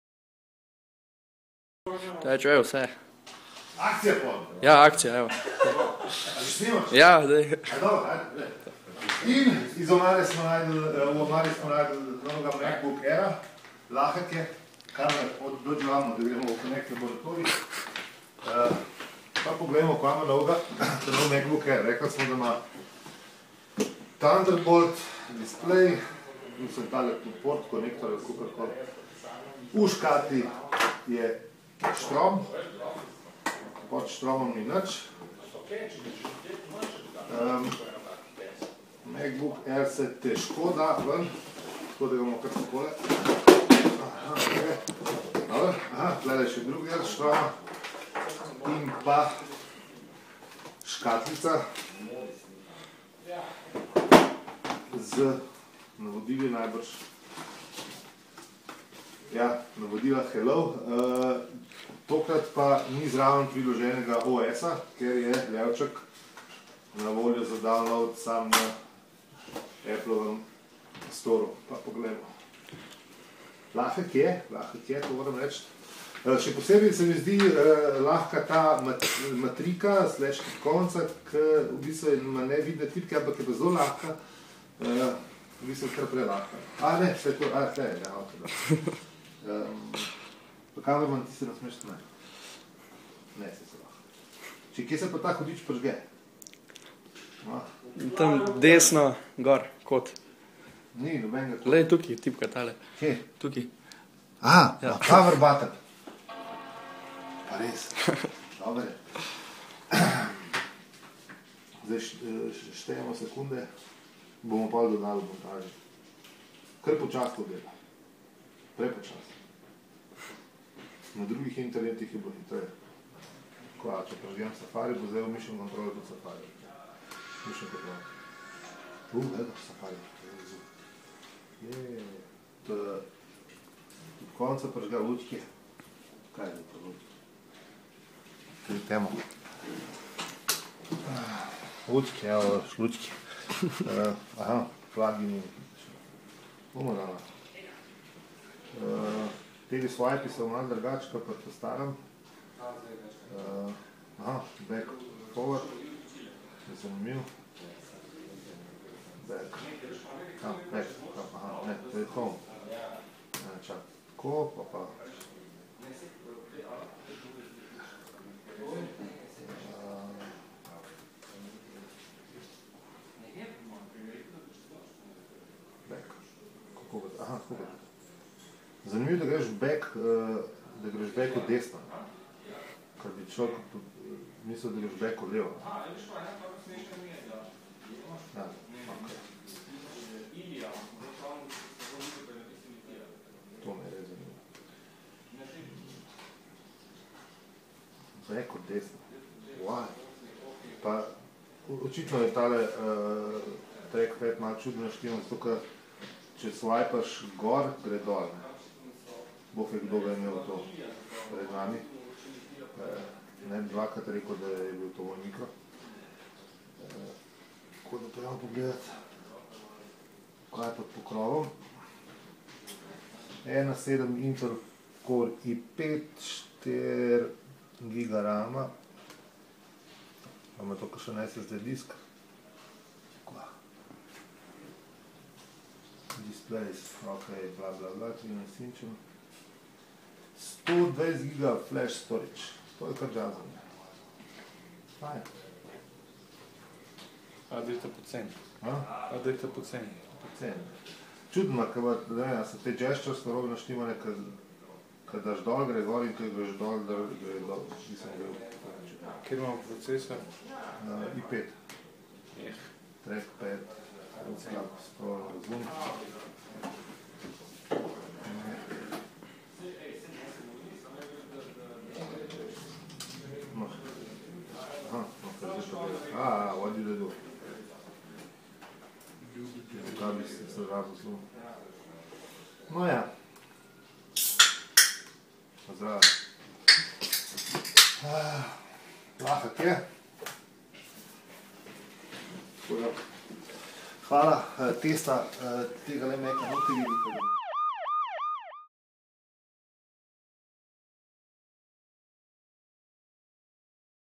Apple MacBook Air - 2011 Corei5, 4GB RAM
Najtanjši predstavnik jabolčne prenosne družine je tu, imamo ga v rokah in predstavljamo vam vse novosti, s katerimi so ga tokrat naphali pri Applu.